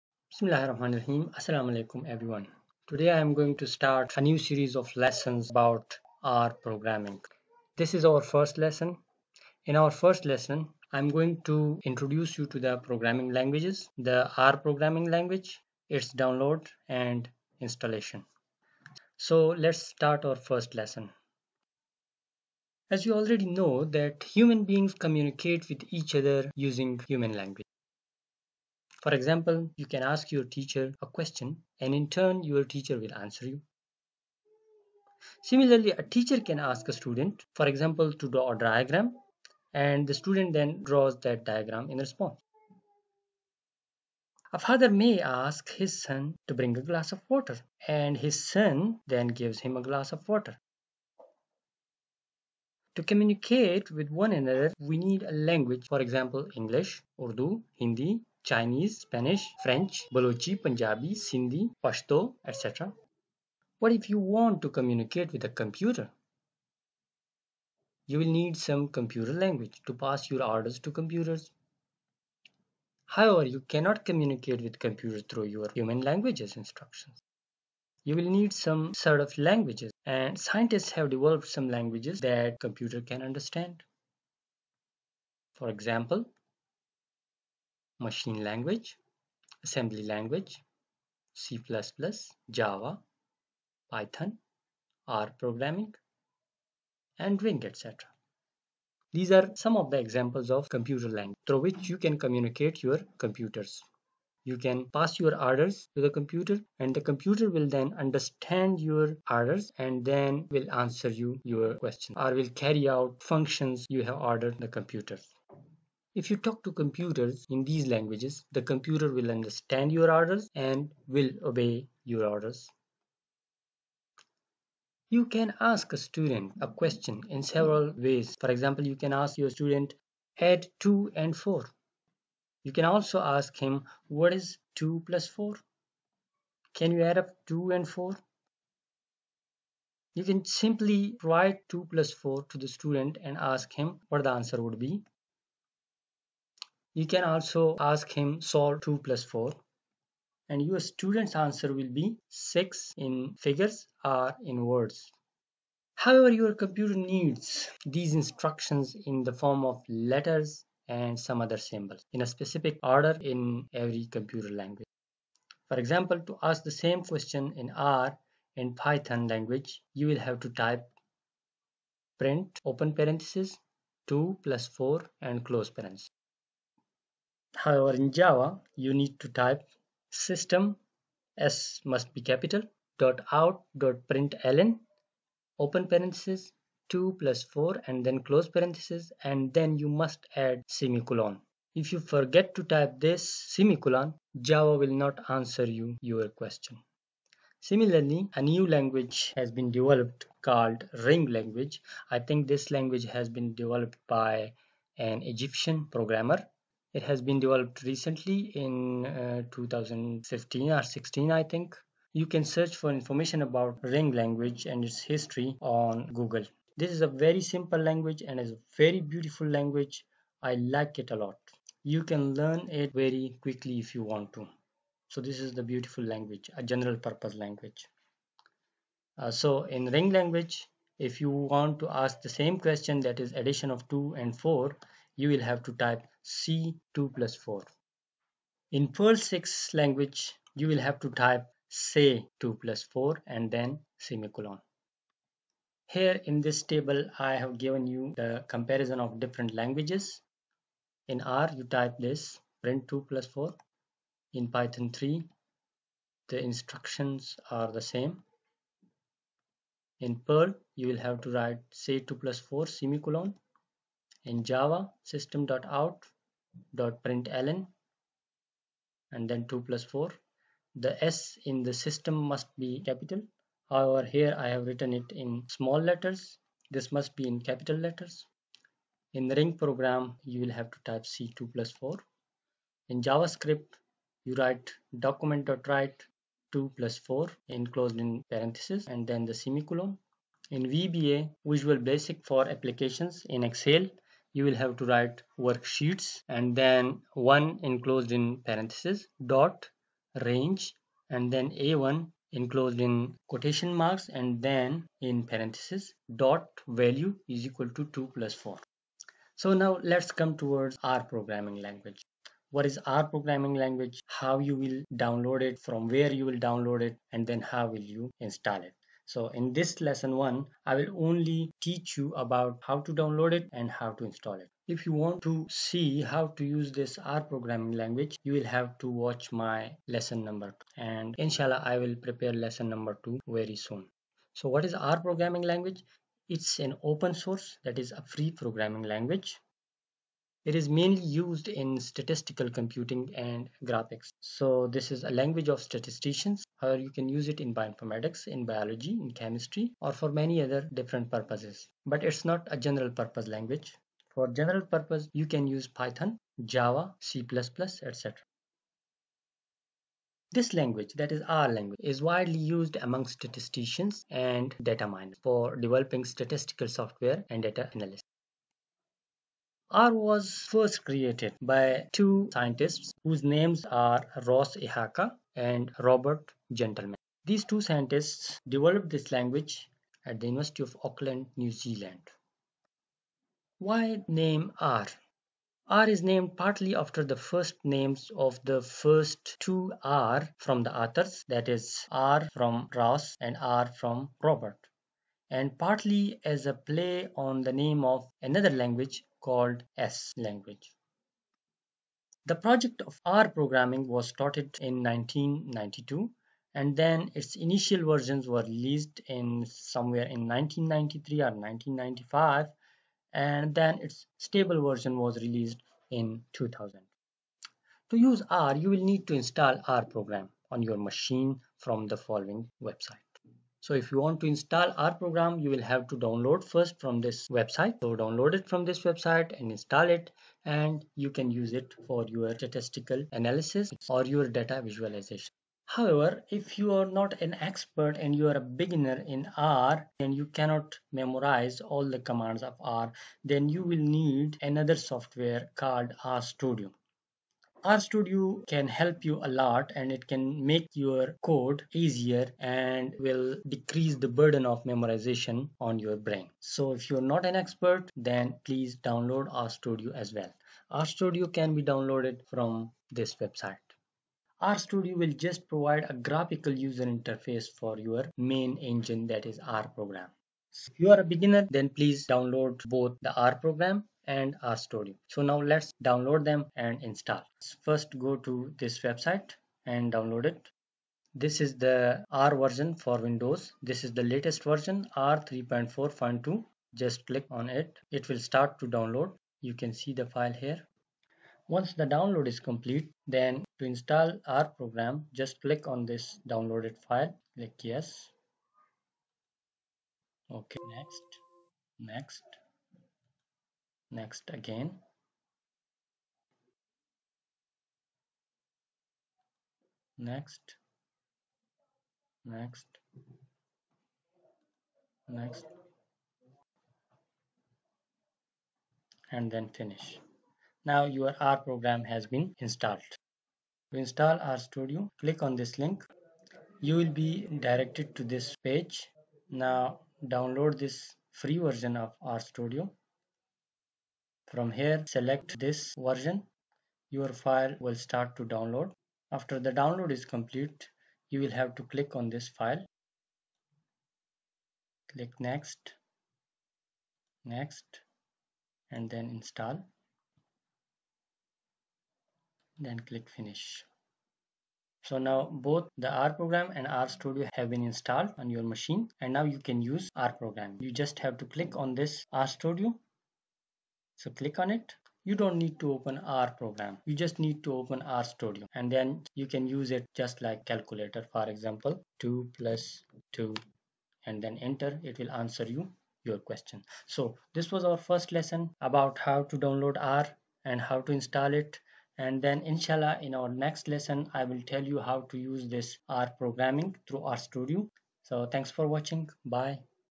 Learn R | Introduction, Download and Installation || Lesson One
Best for entertainment, tourism, picnics, sports, comedy, music and more: @RoshanAliExploration
To learn R, Please watch the series of videos I am uploading about R. My next video is about R as a simple calculator. In this lesson video you will learn about how to download, Install and use R for the first time. Also we will install RStudio.